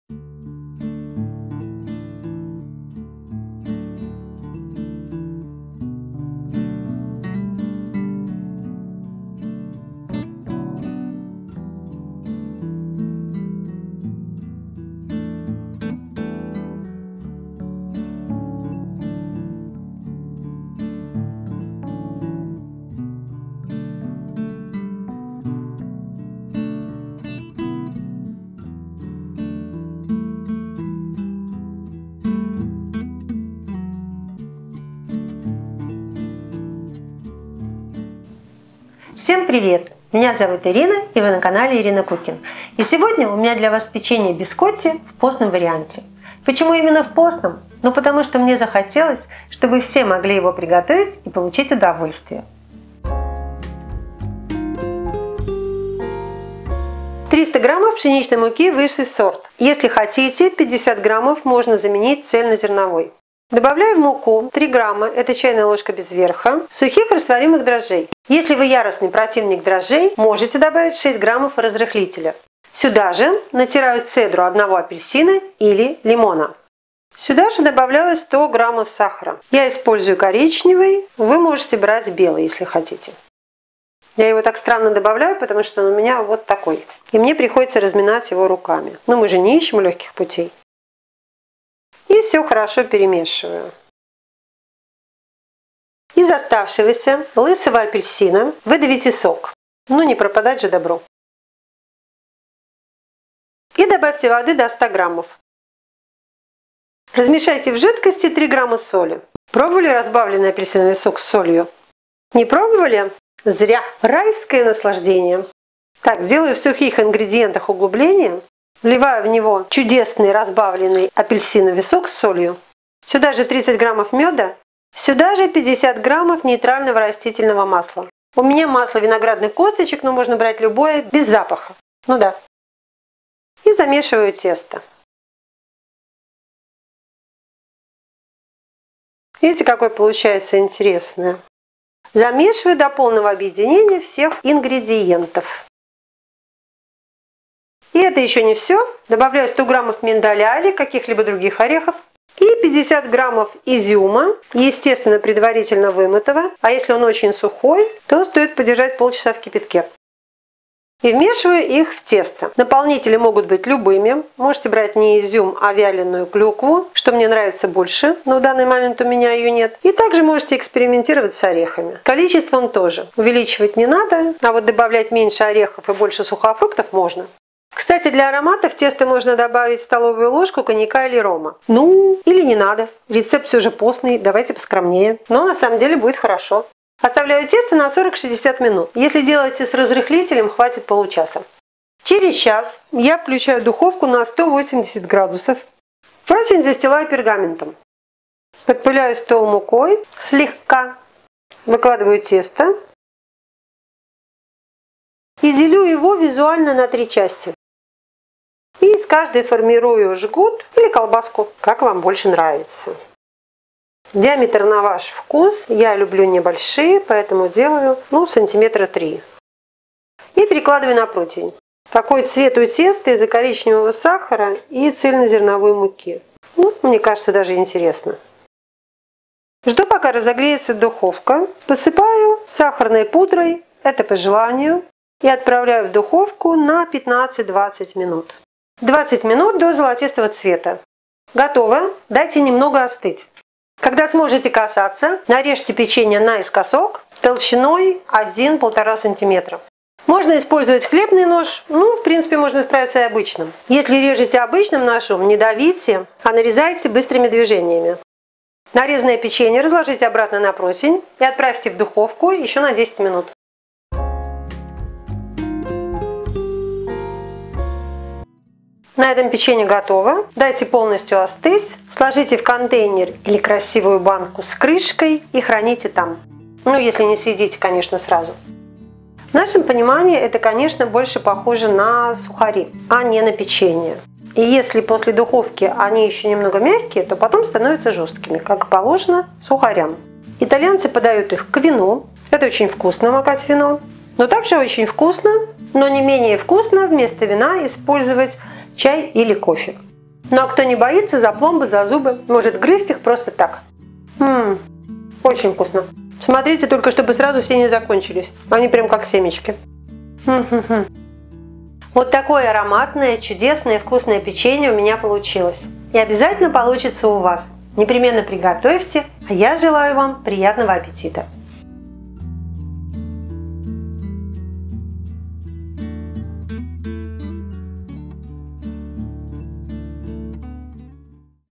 Итальянское Печенье Бискотти Постное Рецепт без яиц и молока
В видео я показываю, как можно приготовить печенье Бискотти в постном варианте. Это рецепт подойдет для любого меню, в том числе веганского.

Рецепт печенья Бискотти:
300 г мука (или 250 г в/с + 50 г ц/з)
3 г сухие инстантные дрожжи (или 6 г разрыхлитель)
100 г сахар (лучше коричневый)
3 г соль
30 г мед
цедра 1 апельсина
100 г сок апельсина + вода
50 г нейтральное растительное масло
100 г орехи
50 г сухофрукты